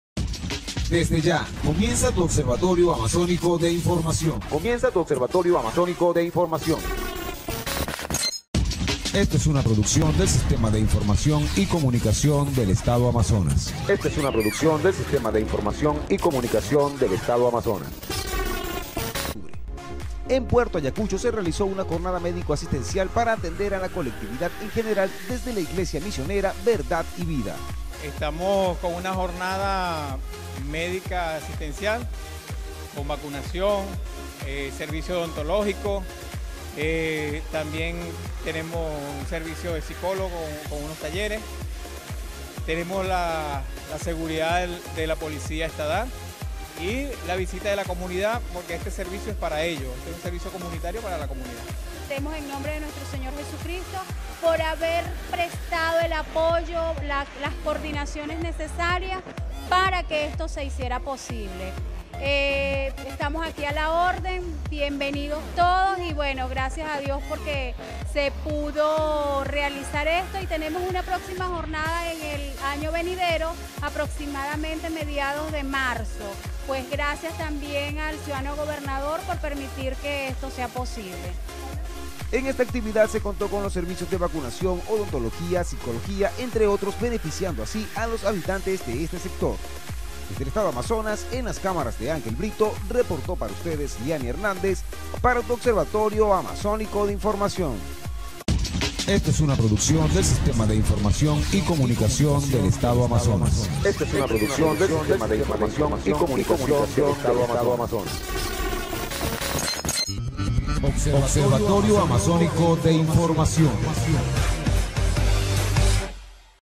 Amazonas | Jornada Médica Asistencial en la Iglesia Misionera Verdad y Vida en Amazonas
Amazonas | Amazonenses recibieron Jornada Médica Asistencial en la Iglesia Misionera Verdad y Vida